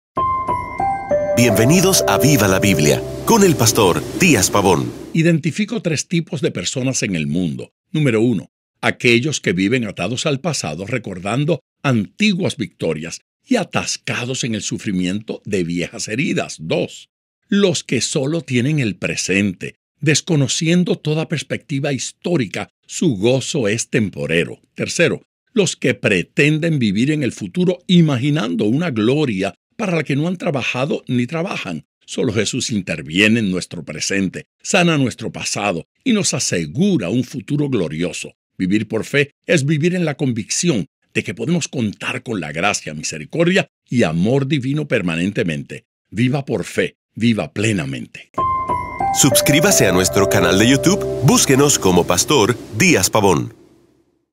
Viva por fe VLB23048
Identifico tres tipos de personas en el mundo: Número uno: Aquellos que viven atados al pasado recordando antiguas victorias o atascados en el sufrimiento de viejas heridas. Número dos: Los que solo tienen el presente, desconociendo toda perspectiva histórica su gozo es temporero. Tercero: Los que pretenden vivir en el futuro imaginando una gloria para la que no han trabajado ni trabajan.
Solo Jesucristo interviene en nuestro presente, sana nuestro pasado y nos asegura un futuro glorioso. Vivir por fe es vivir en la convicción de que podemos contar con la gracia, misericordia y amor divino permanentemente. Viva por fe, viva plenamente.

Por el Dr. Díaz-Pabón, editor general de la Biblia del Pescador

Viva la Biblia 23048